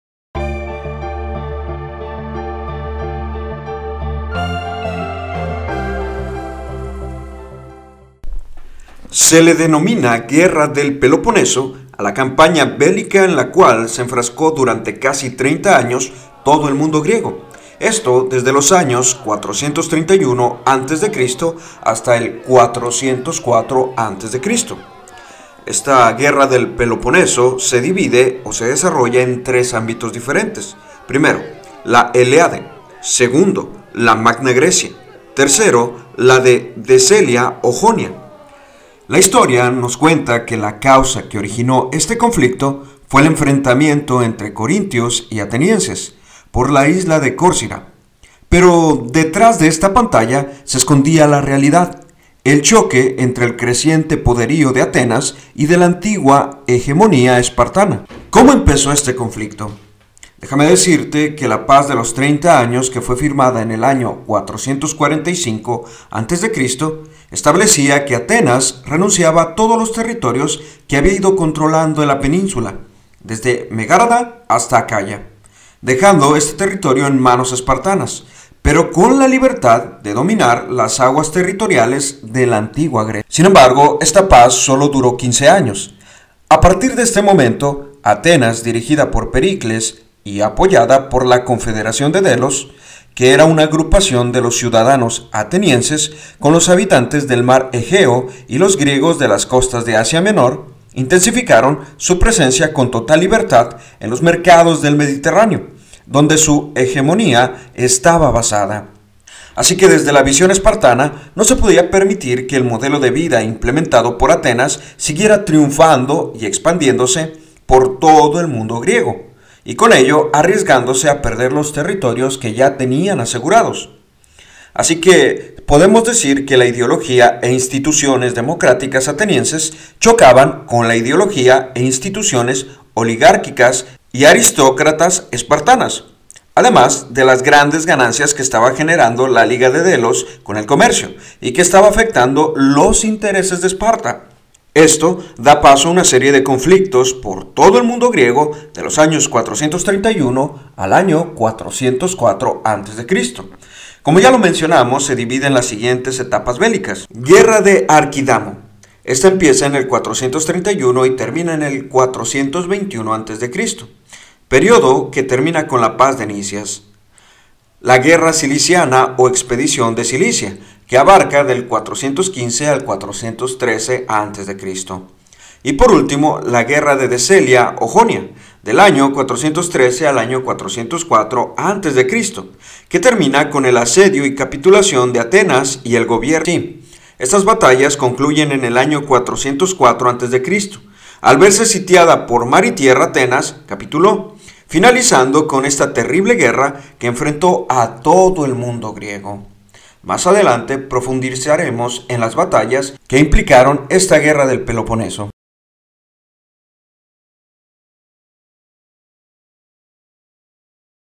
La guerra del Peloponeso
Una guerra que sumió en el caos al antiguo mundo griego durante varias décadas.